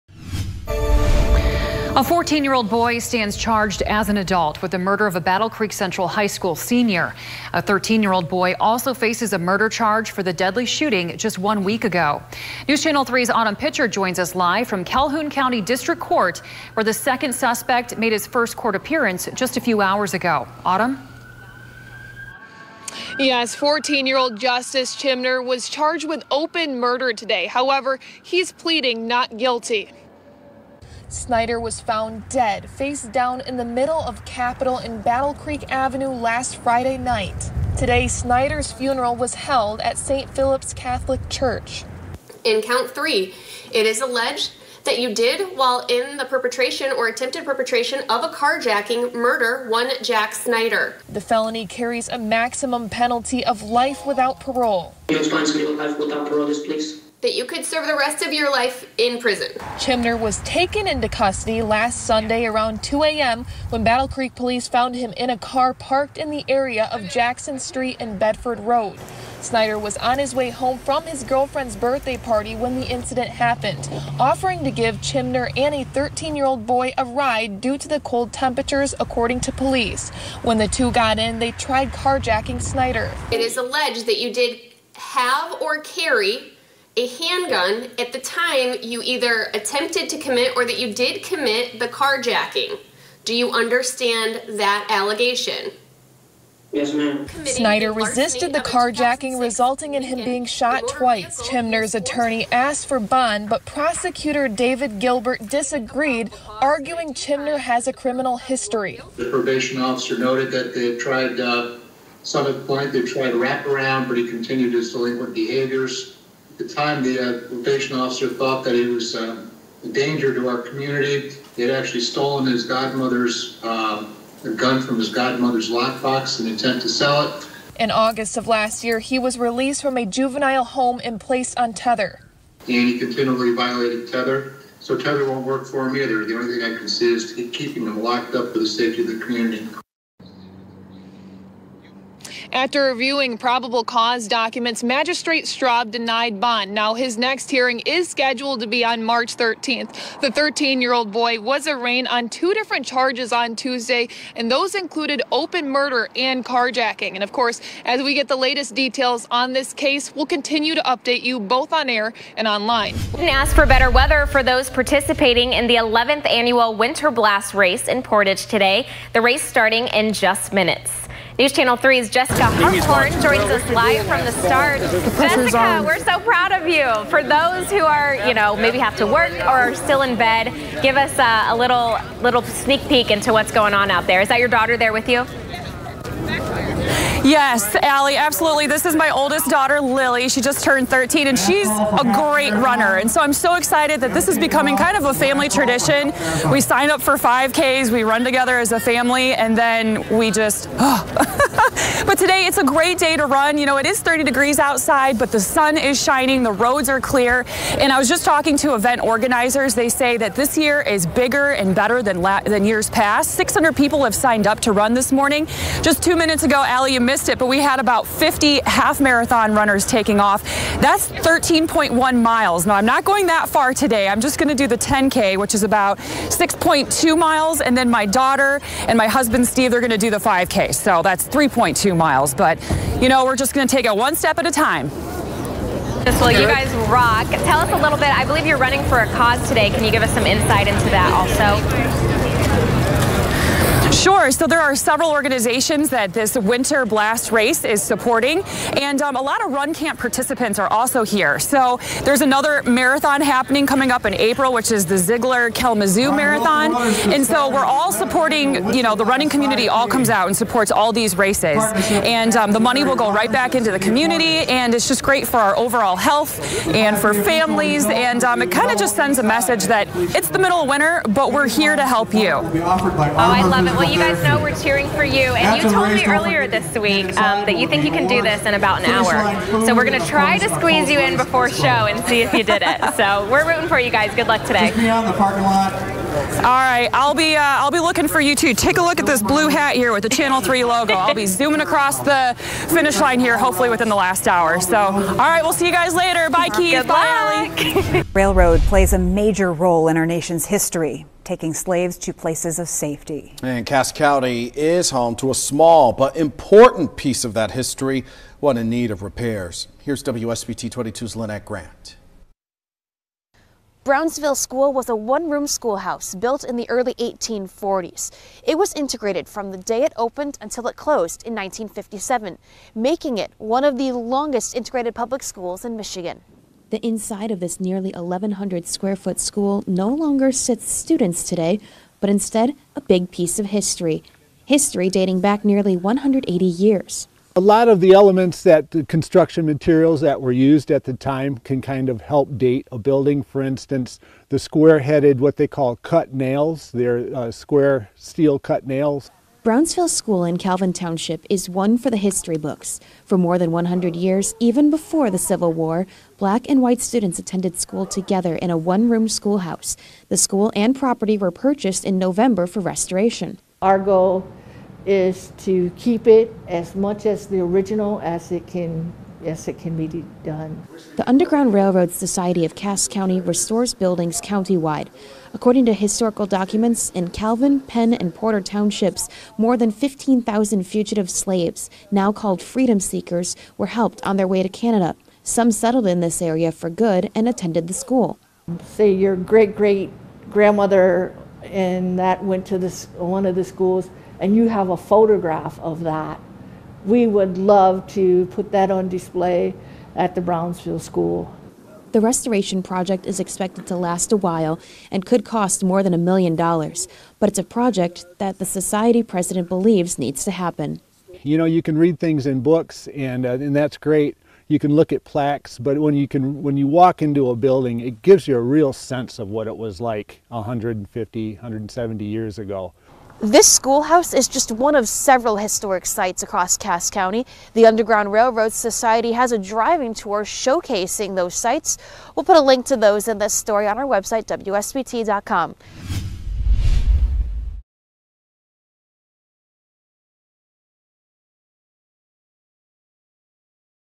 14-year-old Justice Chimner charged in deadly shooting of Battle Creek Central High School student
A 14-year-old boy was arraigned Friday in Calhoun County District Court, accused of shooting and killing of a 17-year-old Battle Creek Central High School student.

Justice Chimner was charged with the following:

Count 1 - Carjacking of 2006 Lincoln with violence
Count 2 - Felony firearm
Count 3 - Felony open murder
Count 4 - Felony firearm
According to his attorney, Morgan Pattan, Chimner pleaded not guilty and asked for a preliminary trial and the court to issue a cash bond.

Calhoun County Prosecutor David Gilbert asked the court for no bond, and said Chimner has a previous history of stealing and attempting to sell a gun.

Plus, Underground Railroad landmark in Cass County in need of repairs, and Jessica Harthorn and family run in 11th annual Portage Winter Blast

0:00 14-year-old charged in deadly shooting of Battle Creek Central High School student
2:43 Underground Railroad landmark in Cass County in need of repairs
4:58 Jessica Harthorn and family run in 11th annual Portage Winter Blast